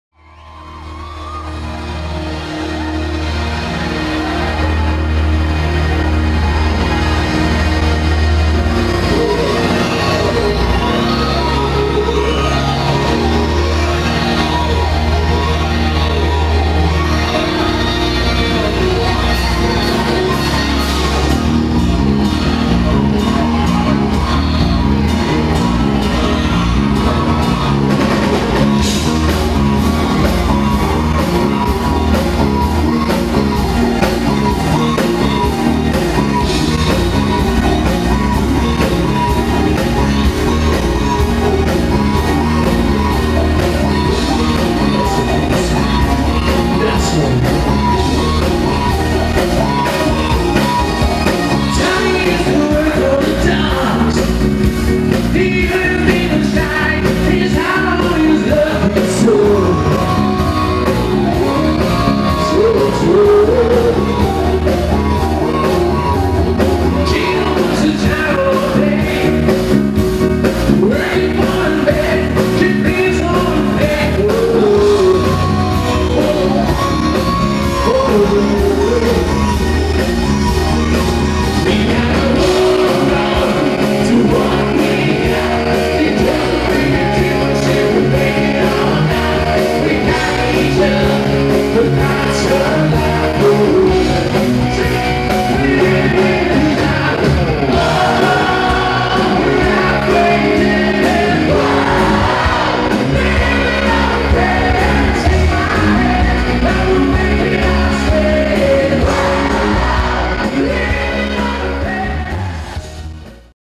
Bon Giovi - Coming To The Kings Hall, Herne Bay Sat 28th March
This will be one of the top performances at The Kings Hall in 2015 as Europe's top tribute to Bon Jovi - Bon Giovi, grace the Kings Hall stage on Sat 18th March.

Expect Jovi after Jovi anthem and a packed house  on a night of breathtaking pure rock.

Tickets £15, available from The Kings Hall, Peters Produce, William Street, Kids Korner, Mortimer Street, or online here: